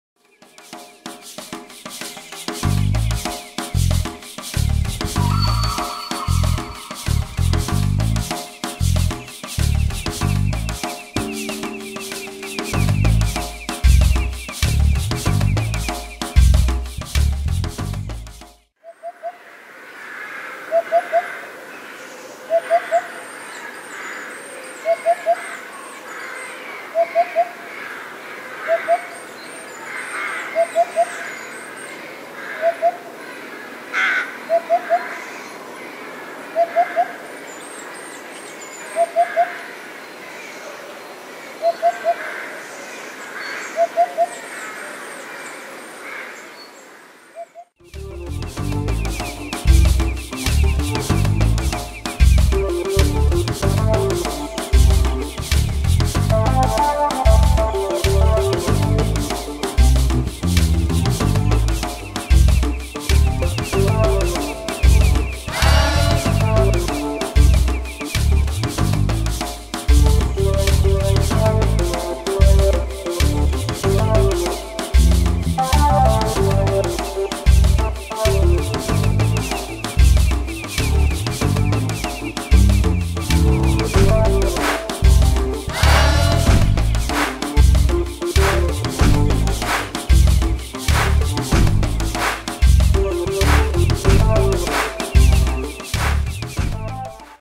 CraftRootz - Hoopoe wire sculpture
Handgemaakt in Zuid-Afrika - prachtige draad beeld met glaskralen in de vorm van een Hoepoe vogel.

Crafted by hand in South Africa - gorgeous wire and glass beaded statue in the form of a Hoopoe.